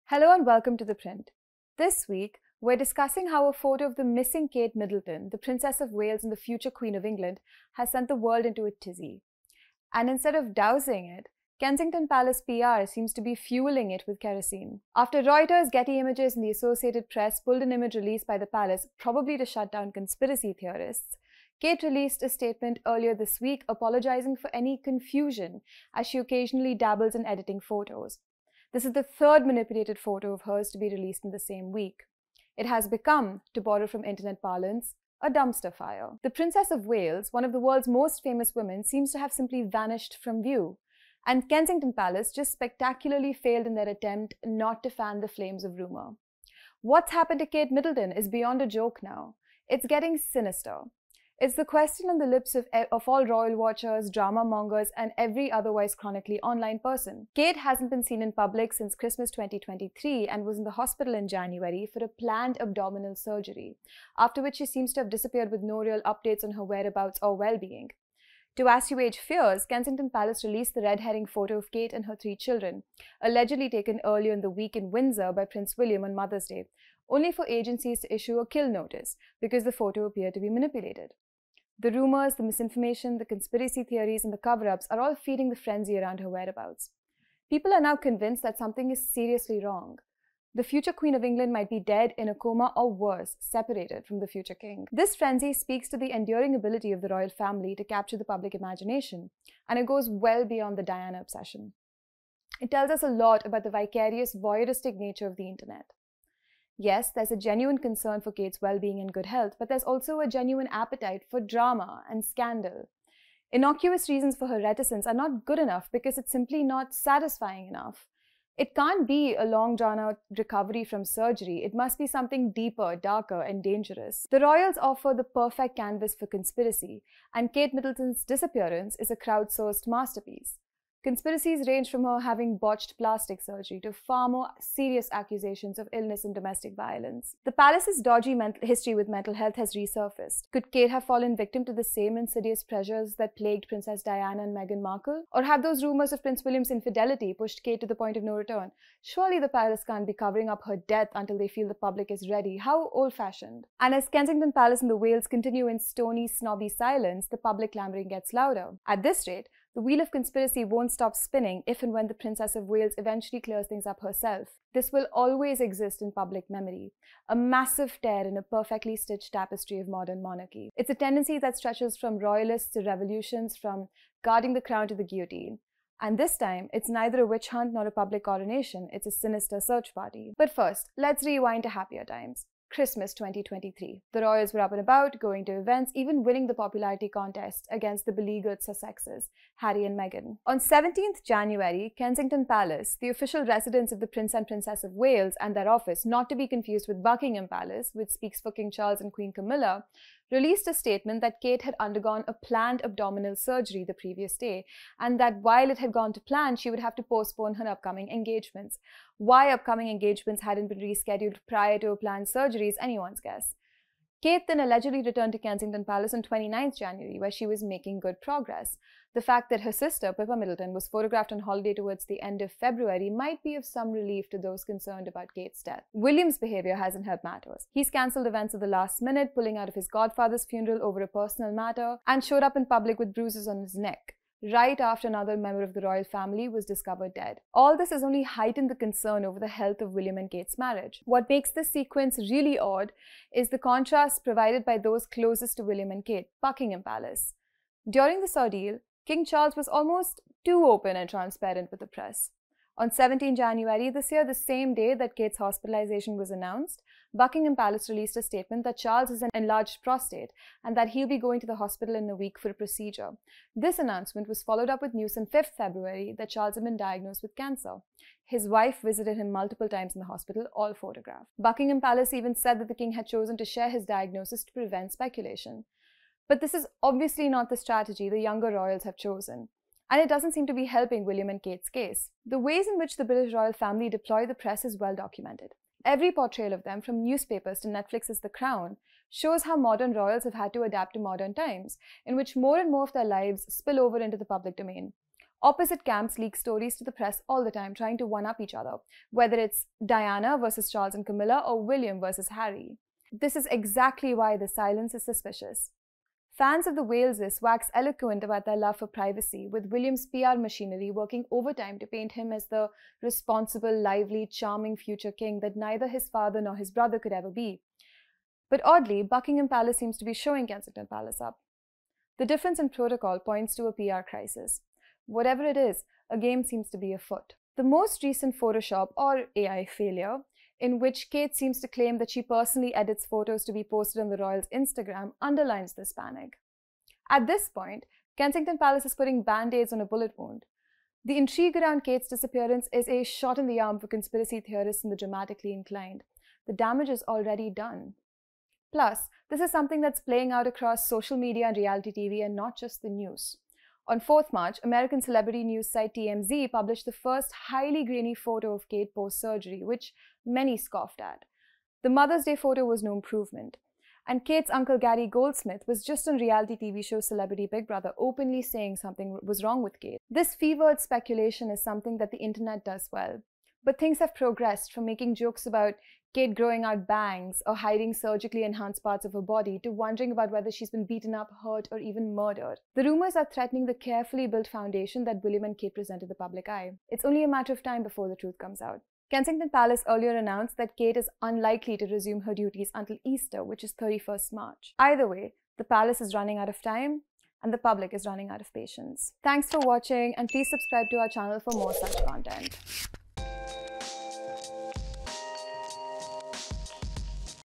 ‘Missing’ Kate Middleton isn’t just a PR crisis — it has sparked a sinister search party
‘What’s happened to Kate Middleton’ is beyond a joke now — it’s getting sinister. It’s the question on the lips of all royal watchers, drama-mongers, and every otherwise chronically online person. This frenzy speaks to the enduring ability of the royal family to capture the public imagination, and it goes well beyond the Diana obsession. It tells us a lot about the vicarious, voyeuristic nature of the internet.